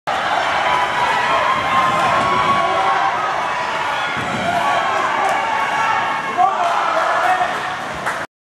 Arrêt de Noah P Moore 45 sur Yan Philippe Mauger 17
Le rapide Yan-Philippe Mauger (17) s'échappe seul sur Noah P.Moore qui réalise l'arrêt sans accorder de retour. Match nul 1 à 1 vs Bélanger-Couture de G-Rivière 23 nov. 14.